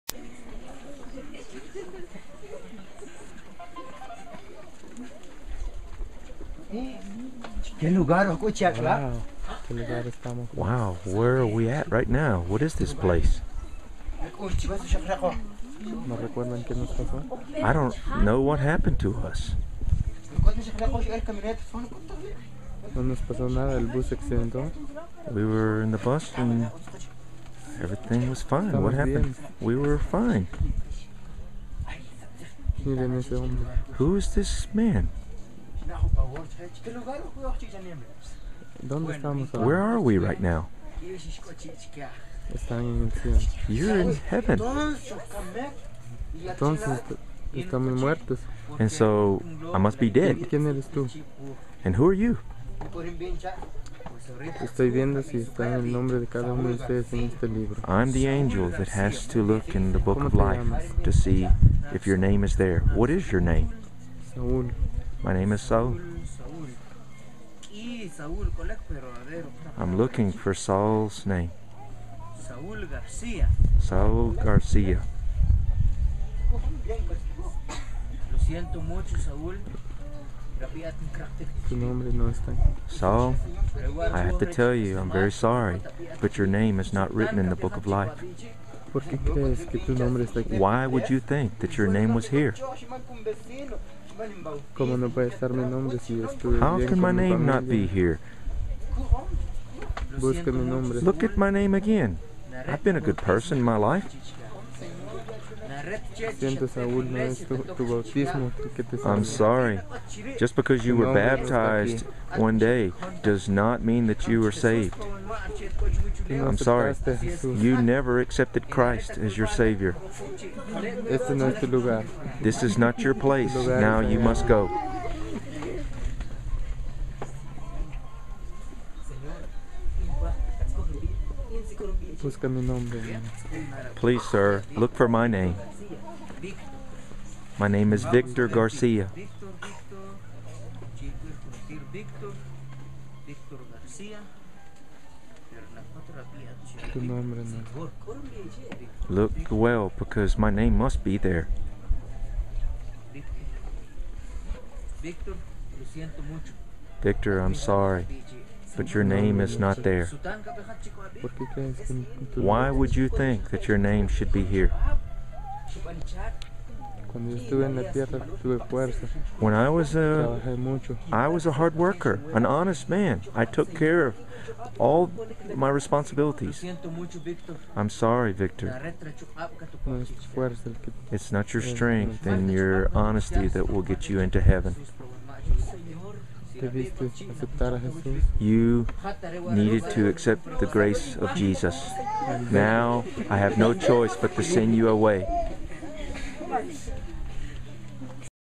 Are You Sure? p2.mp4
Part 2 of the Drama presented in Chuchuca during the 2011 Campaign